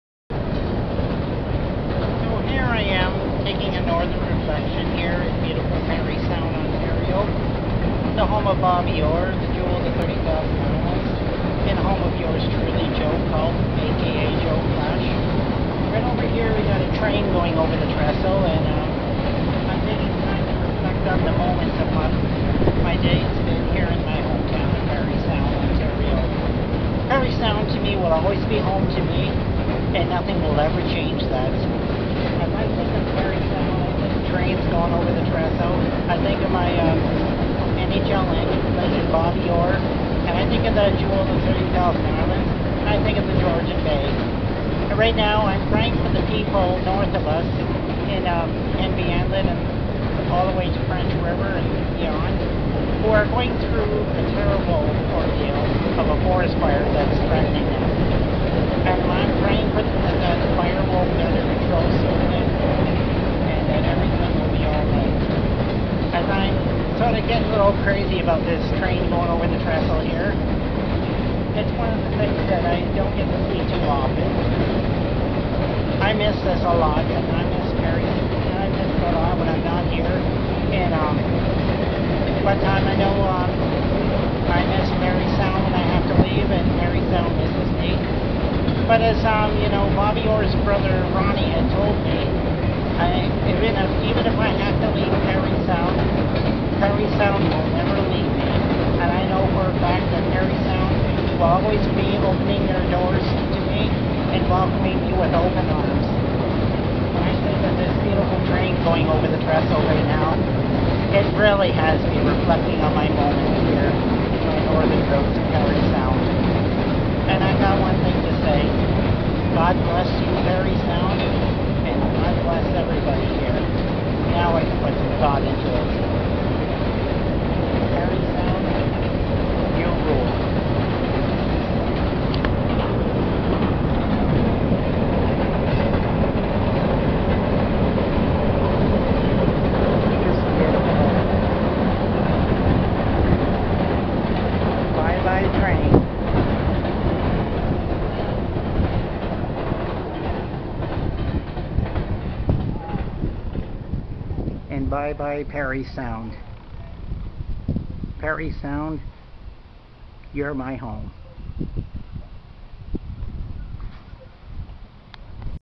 My Parry Sound Moment/Train (August 5, 2018) brought to you by Joe Culp (aka Joe Flash)
Live Video of my giving My Hometown perspective of Parry Sound Ontario on my visit in Parry Sound Ontario on a Hot Humid Summer Day of 2018 and I figured while I was sitting under a shady art station to do a live show with the CN Intermodal Northbound Train crossing over the Trestle which was something I had always wanted to do. And thankfully I had the chance. I was giving my final thought before it was time to leave me my beloved Hometown again. Its too was my way of saying Parry Sound is Open after a wrong News story about parry Sound being under evacuation which was never cause of a Forest Fire unfolding in Henvey Inlet known as Parry Sound 33. Right now my Prayers are going out to those affected by it and to all the Firefighters who are working long and hard to get that Fire under control.